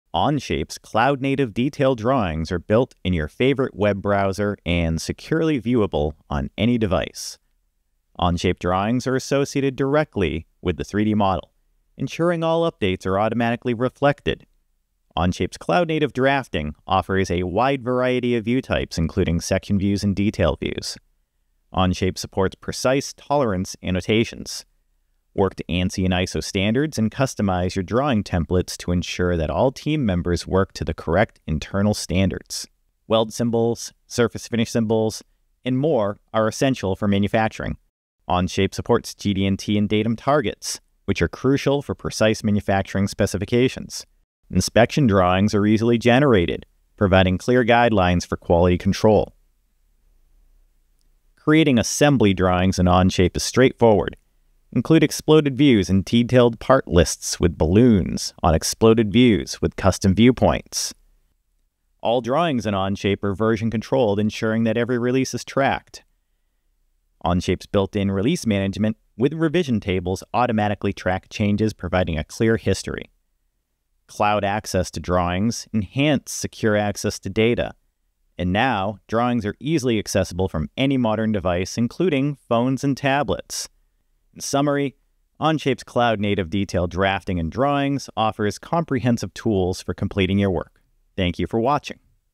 Onshape Drawings
Onshape's cloud-native detail drawings are built in your browser and accessible on any device with drafting features like section and detail views, precise tolerance annotations, GD&T support, and customizable templates for ANSI, ISO, and internal standards. Generate inspection and assembly drawings with exploded views, part lists, and revision tracking through Onshape's built-in release management. Secure cloud access ensures your drawings are always viewable, even on phones and tablets.